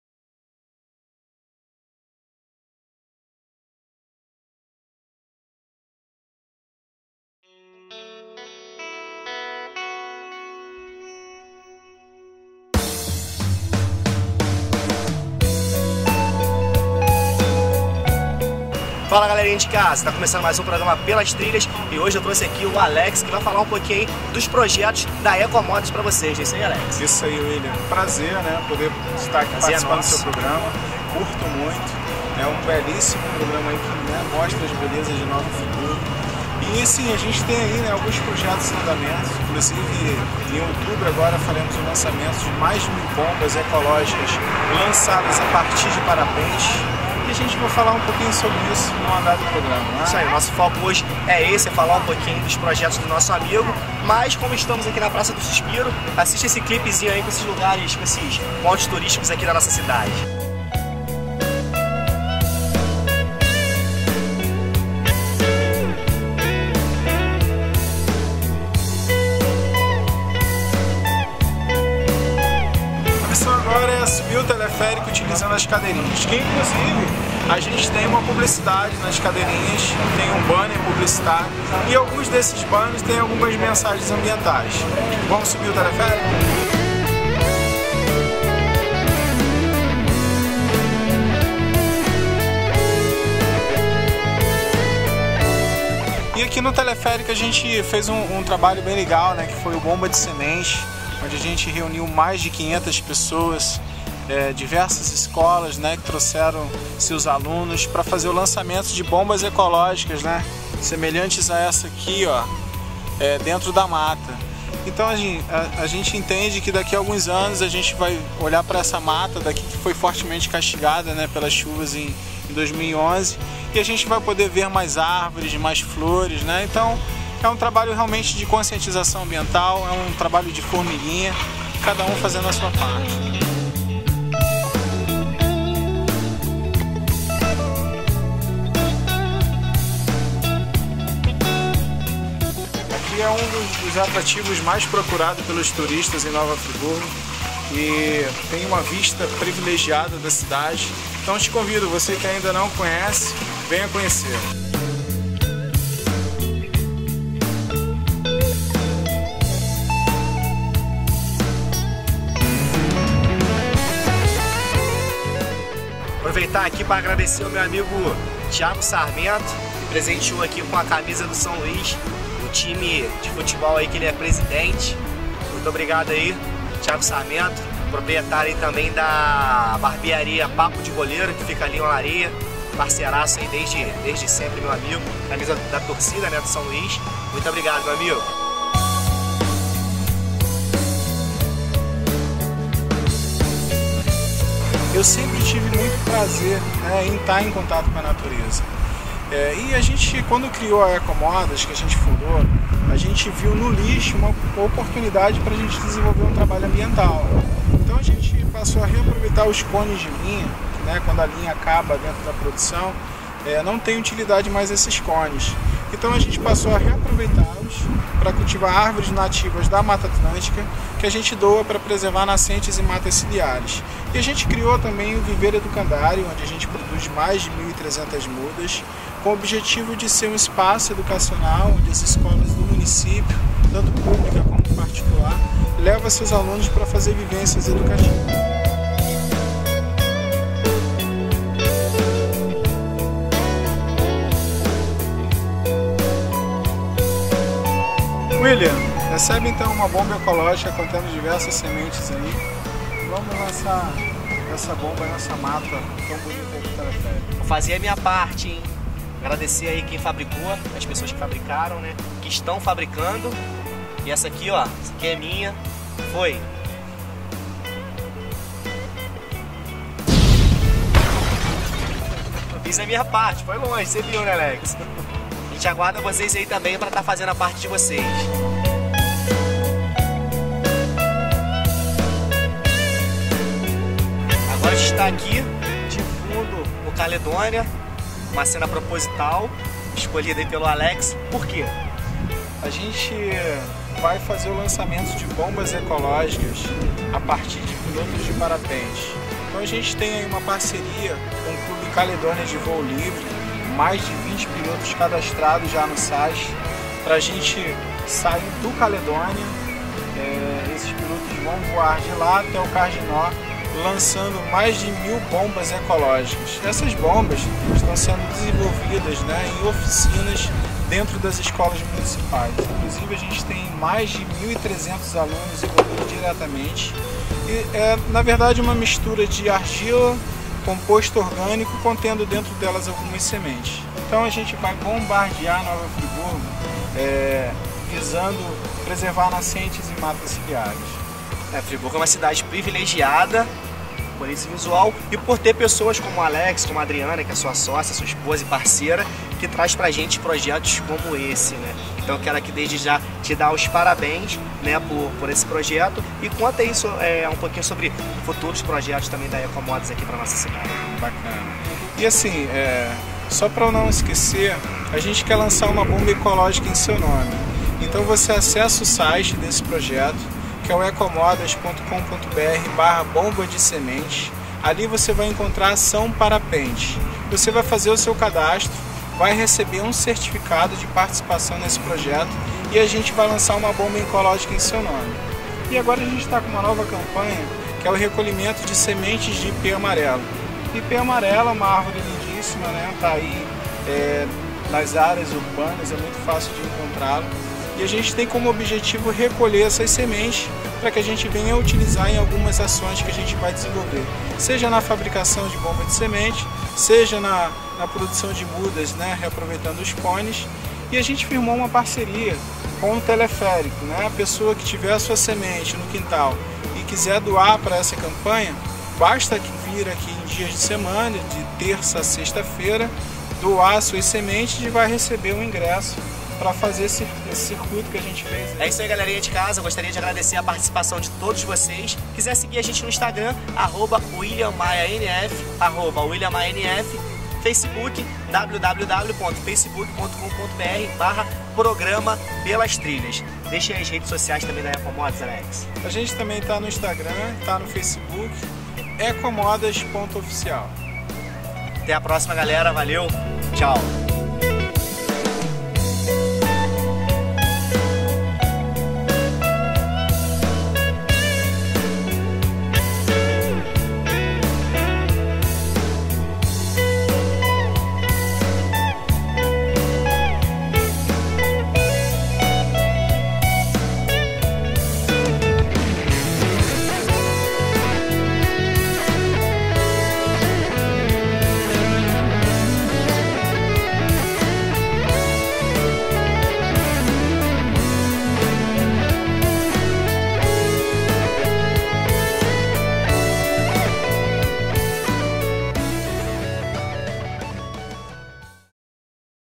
Subindo o Teleférico com a EcoModas
Subimos o Teleférico com o responsável pela Ecomodas, empresa parceira da natureza. Vale a pena conferir.